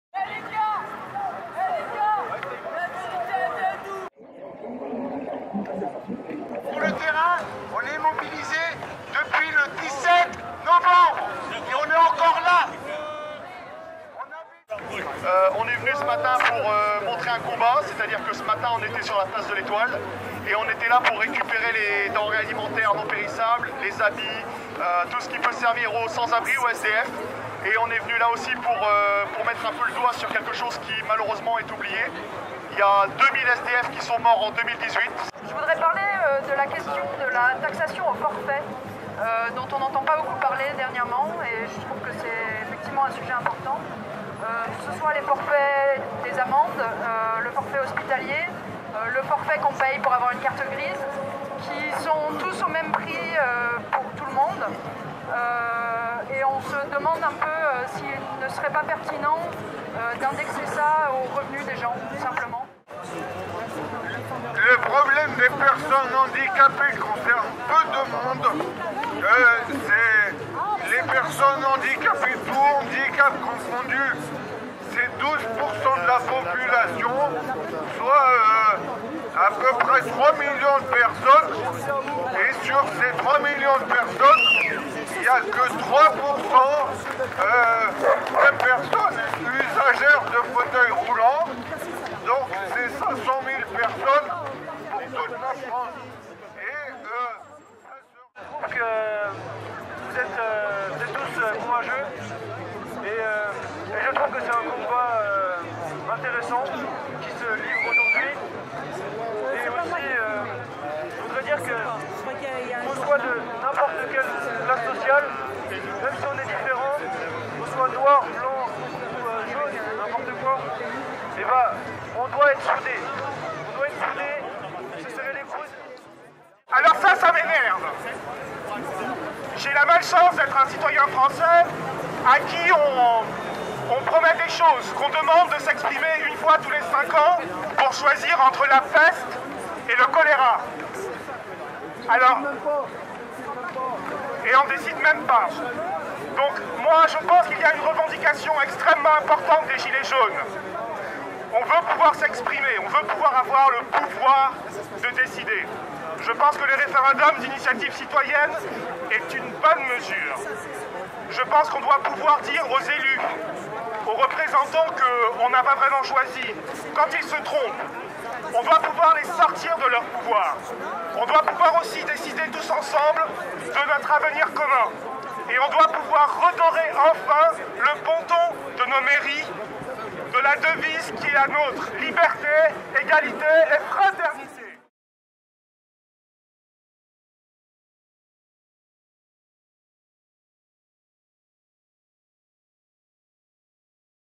La vérité est à nous (nuit jaune) - 7 février 2019 (PARIS)
Compte-rendu vidéo de la journée du samedi 9 février 2019, place de la République, aux pieds de Marianne. Échanges, prises de parole, gilets jaunes ou non, la citoyenneté s'est un instant ravivée.
Samedi 9 février 2019 - Place de la République - Paris